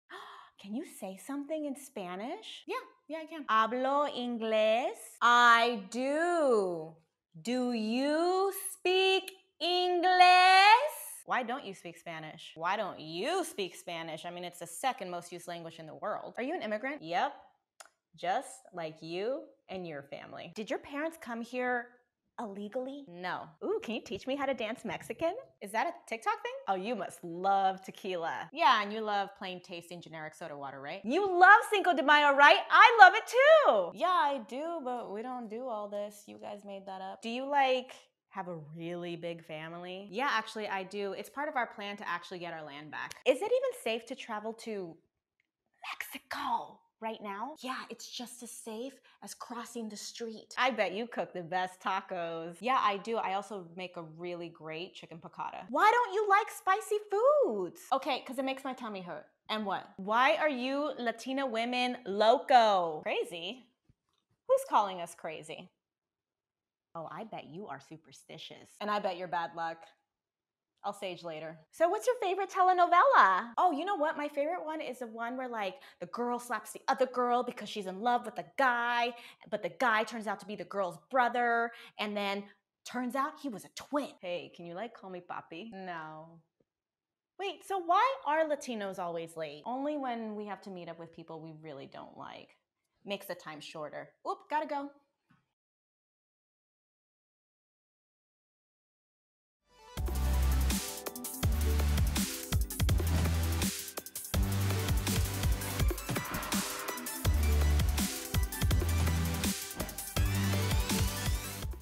Questions to Stop Asking Latinx People with Francia Raisa | Hispanic Heritage Month | Freeform
Racism and microaggressions? Not cute. Francia Raisa handling it? V cute.